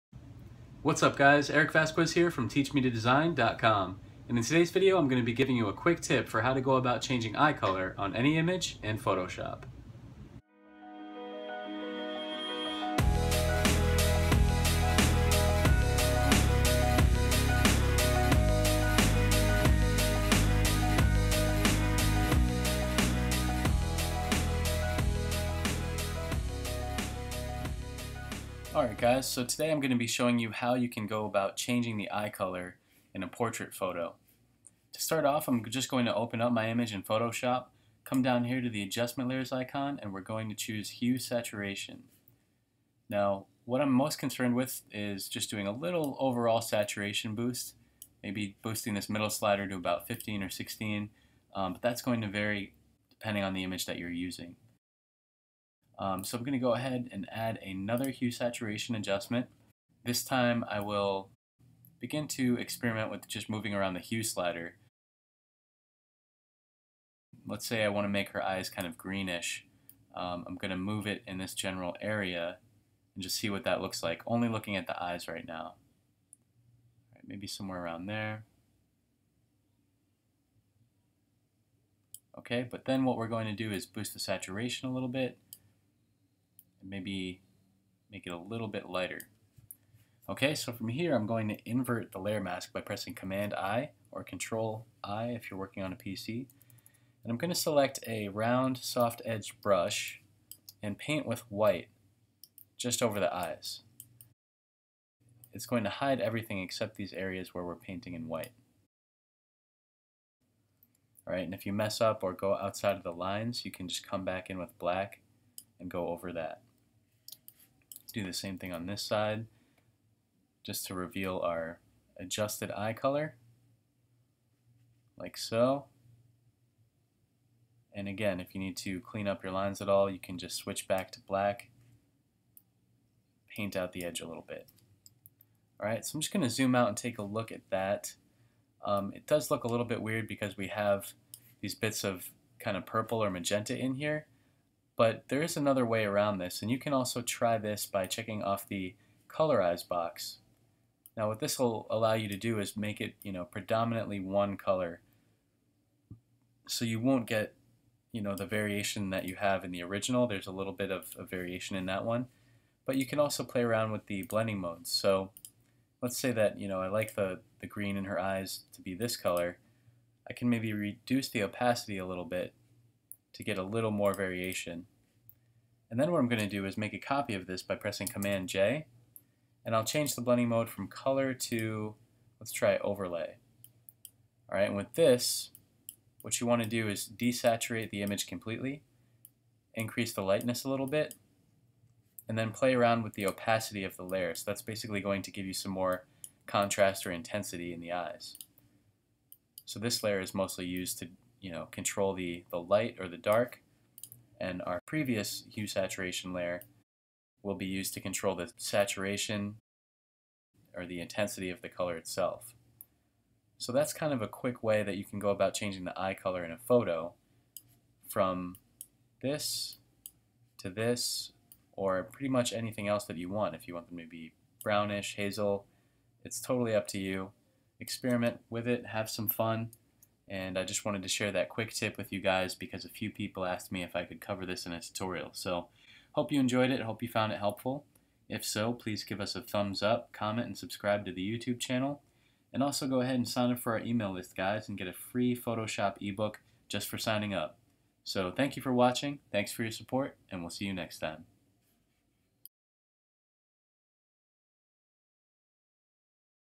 Photoshop Tutorials: Change Eye Color in Photoshop
In this video I will be giving you a quick tip for how to easily change eye color in Photoshop. Although I am demonstrating this technique with an eye, the same approach can be taken if you wish to change the color of other areas of an image as well. We will be using Adjustment Layers and Layer Masks which will give you much more control over the settings and intensity of your colors.